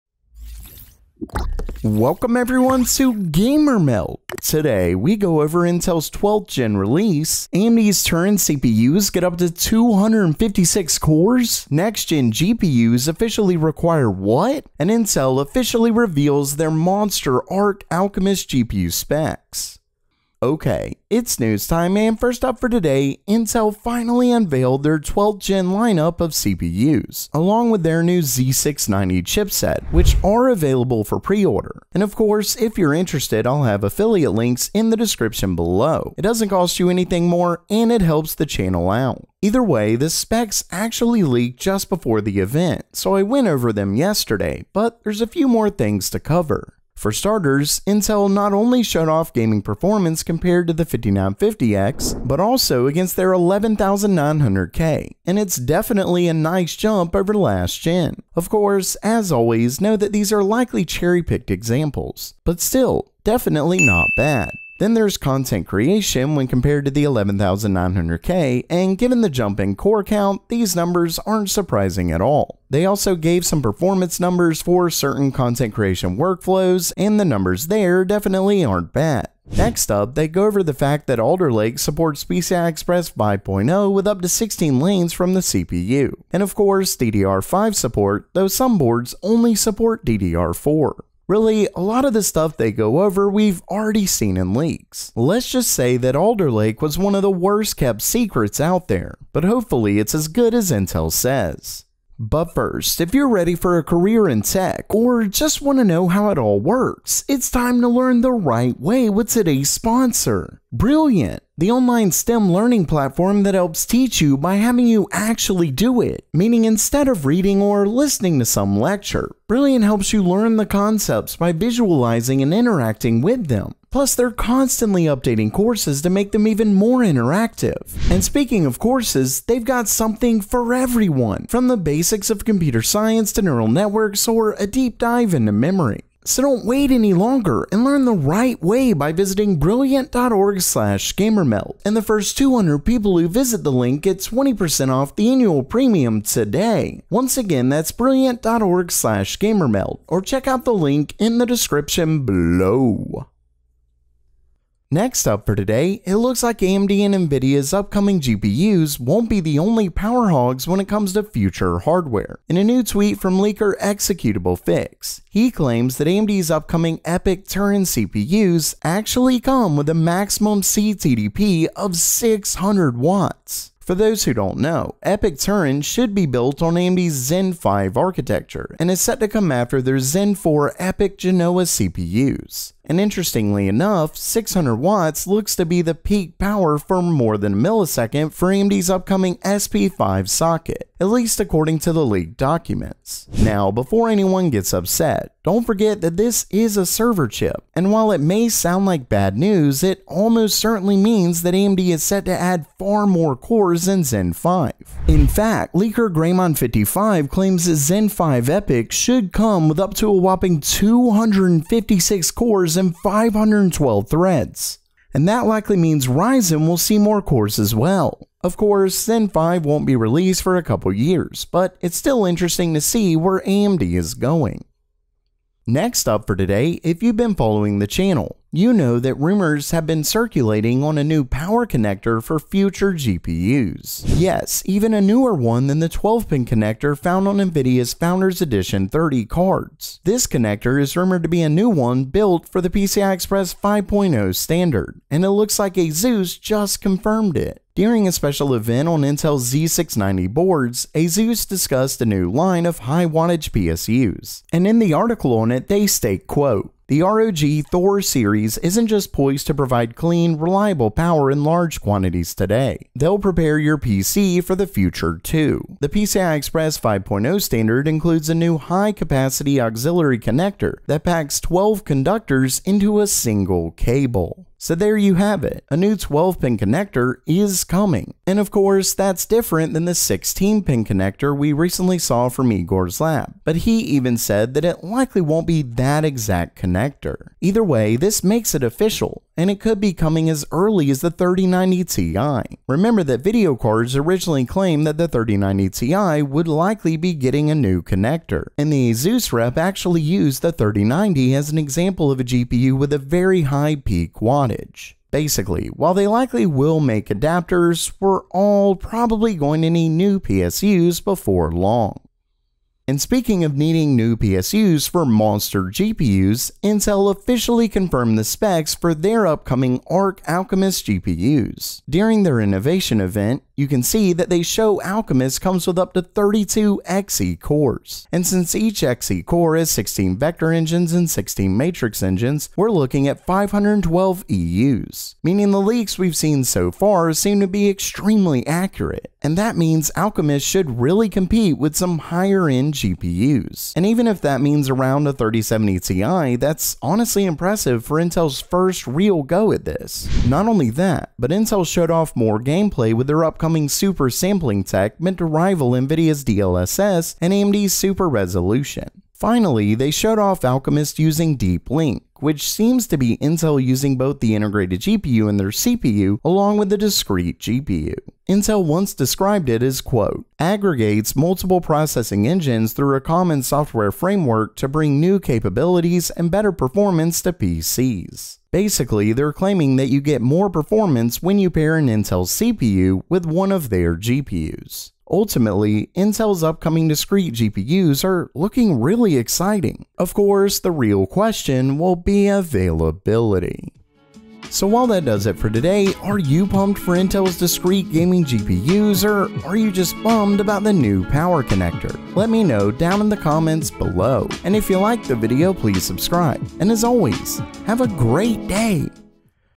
AMD CPUs Are About To Get INSANE!
Intel officially revealed their ARC Alchemist GPU specs, next gen GPUs officially require a new connector, AMD's Turin CPUs get up to 600W and we go over Intel's 12th Gen CPUs!

TIMESTAMPS
0:00 Intro
0:20 Intel's 12th Gen Released
2:34 AMD's Turin CPUs Get Up To 256 Cores
3:45 Next Gen GPUs Require WHAT?!
5:12 Intel OFFICIALLY Revealed Their ARC GPU Specs!